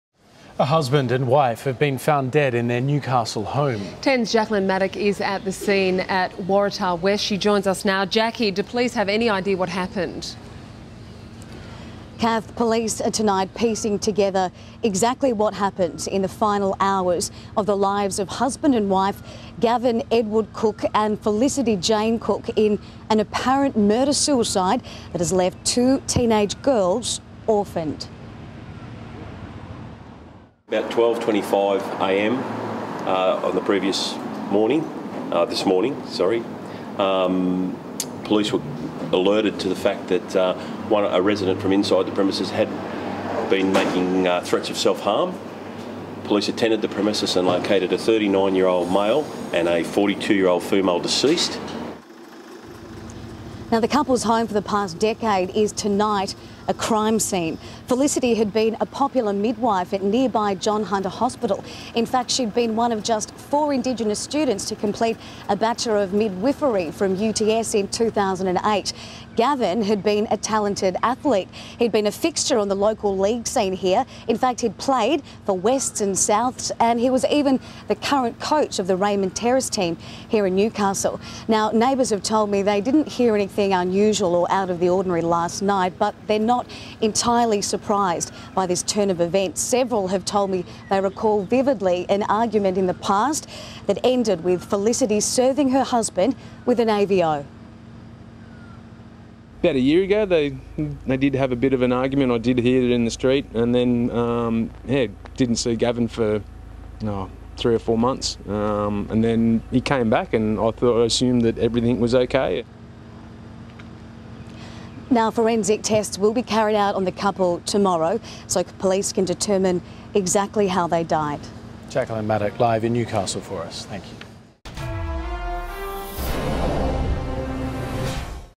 Newcastle couple found dead
A husband and wife have been found dead in their Newcastle home.